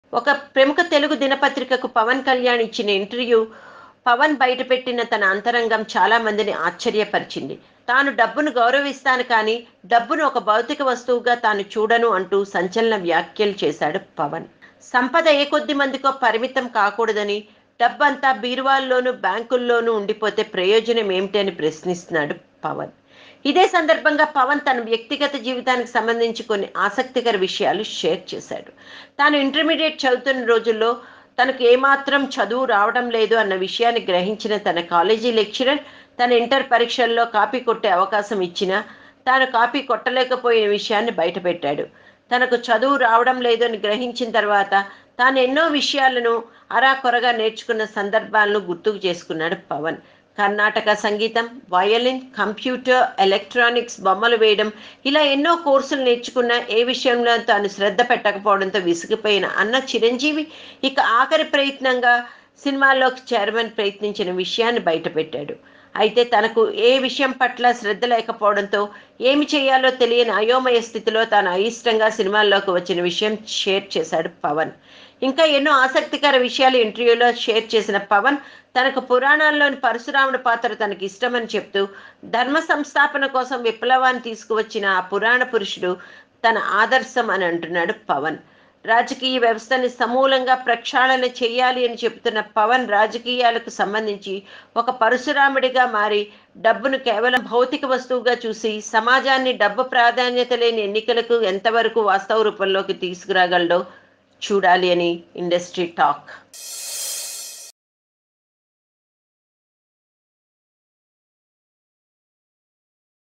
Pawan Kalyan Sensational Remarks On Money Status
Pawan Kalyan who will enter direct politics from next general elections has made sensational comments on money status. Watch this video to know what he said…